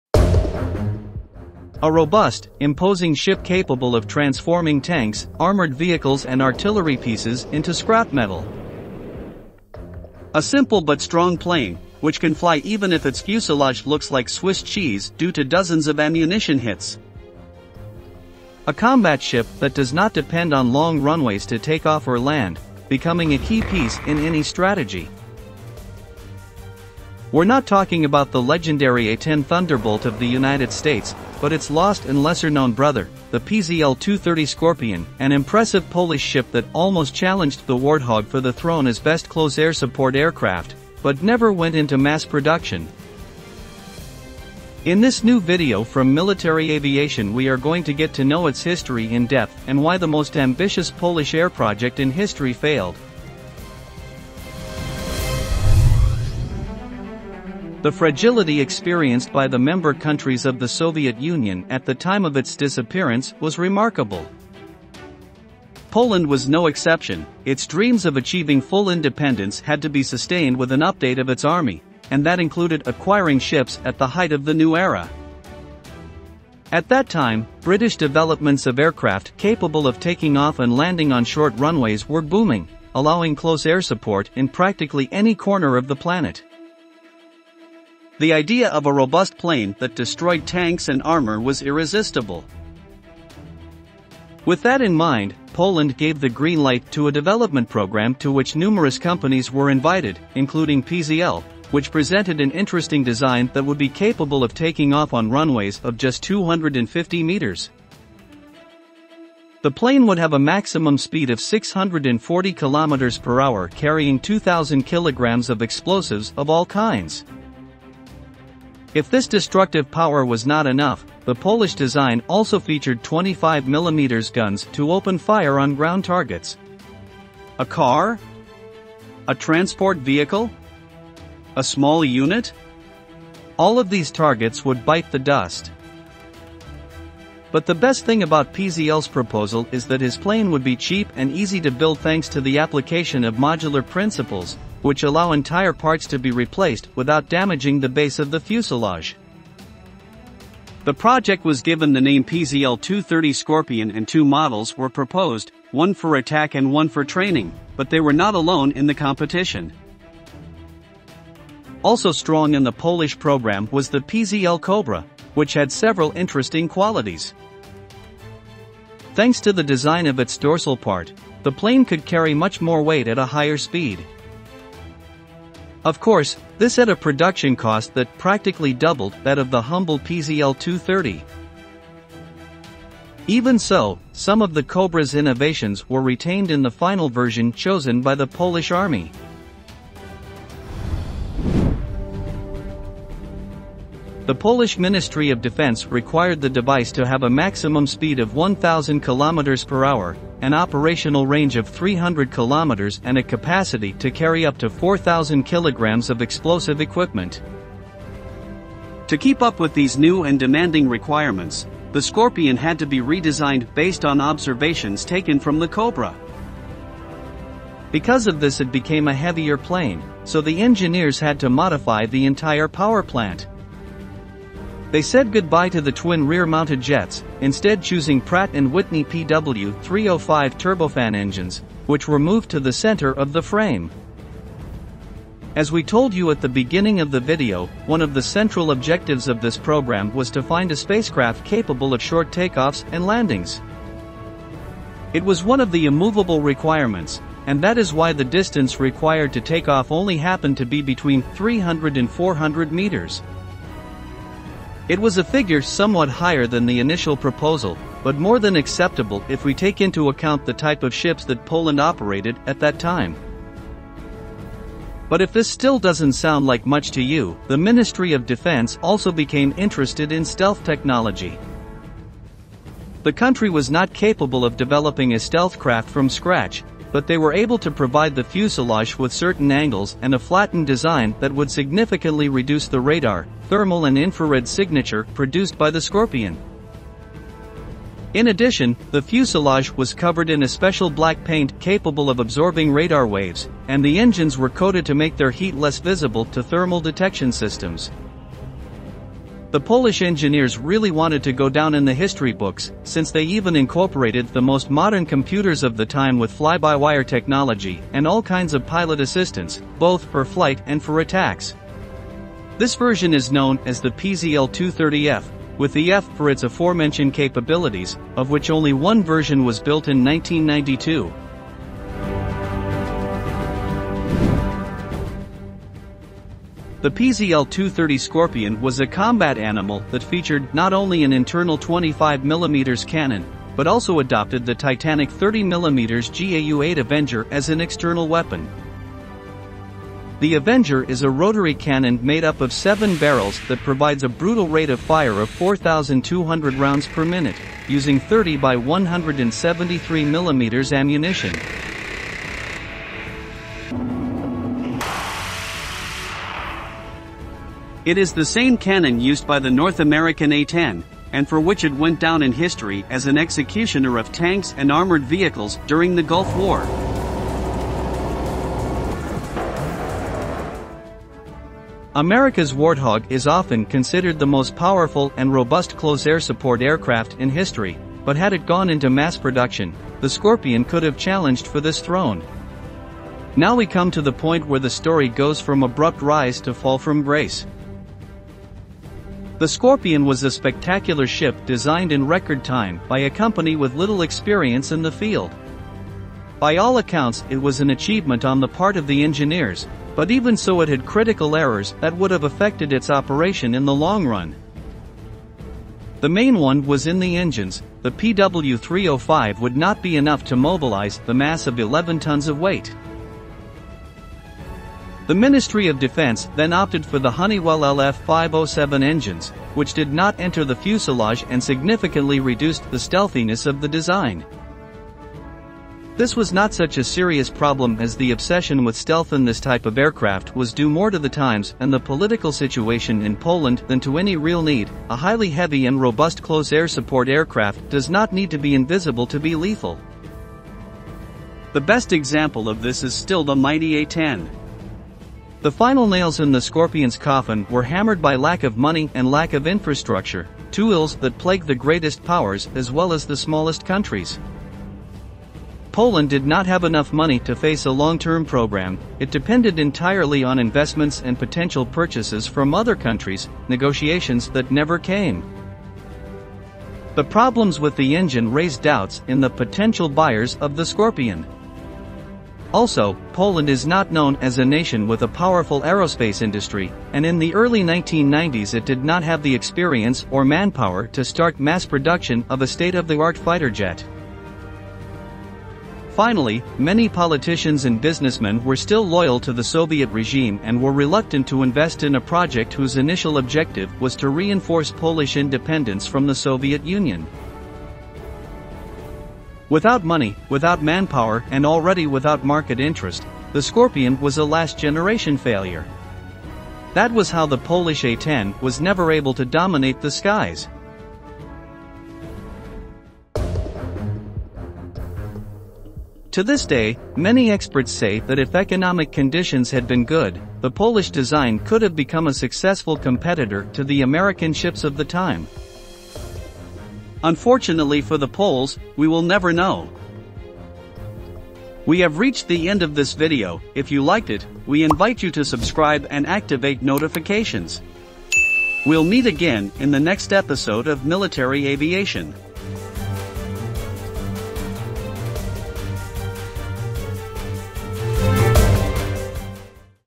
Meet the fearsome Polish A-10: PZL-230 Skorpion
A robust, imposing ship capable of transforming tanks, armored vehicles and artillery pieces into scrap metal. A simple but strong plane, which can fly even when its fuselage looks like Swiss cheese due to dozens of ammunition hits. A combat ship that does not depend on long runways to take off or land, becoming a key piece in any strategy. We're not talking about America's legendary A-10 Thunderbolt, but its long-lost and little-known brother, the PZL-230 Skorpion, an impressive Polish craft that nearly challenged the Warthog for the throne of best close air support aircraft. , but never went into mass production.